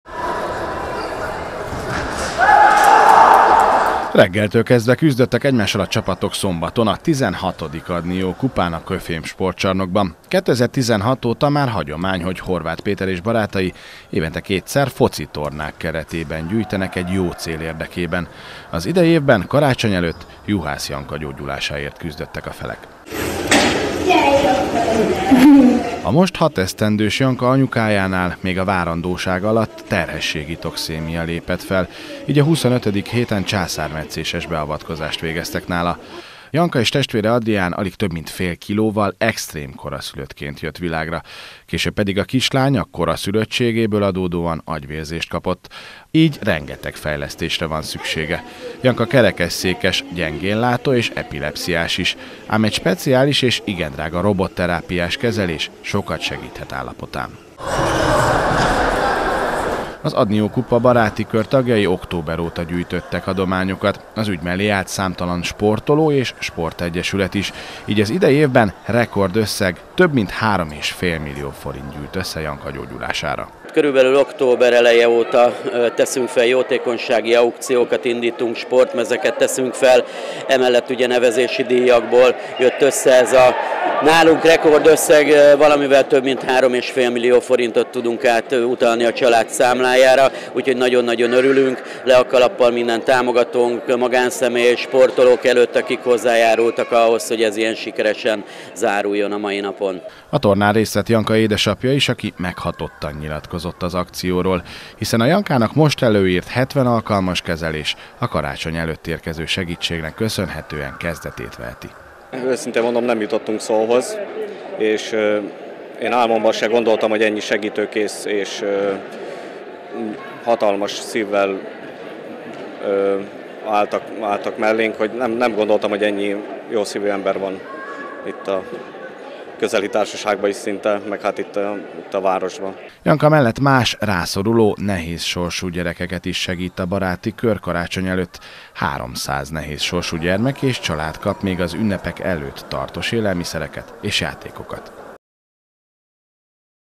REKORD ÖSSZEG GYŰLT ÖSSZE
Rekord összegű adományt gyűjtött az idei évben az Adni Jó Kupa Baráti Kör tagjai. Több, mint 3.5 millió forintot gyűlt össze különböző aukciókból és az év végi Adni Jó Kupa nevezési díjaiból, a hatéves Juhász Janka gyógyulására. Az baráti kör tagjai emellett további 300 családnak segítenek az ünnepek előtt.